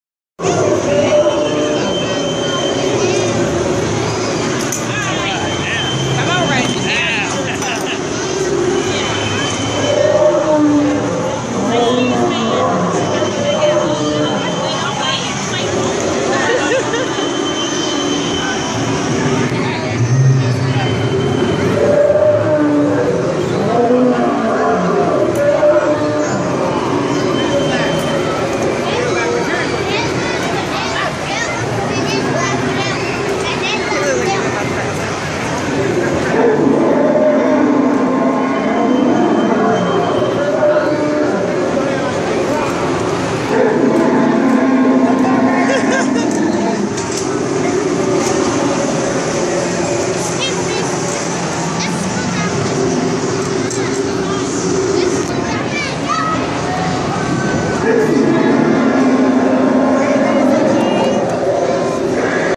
Lucian riding a triceratops
Little man riding a Triceratops at the Jackson MS dinasour exhibit.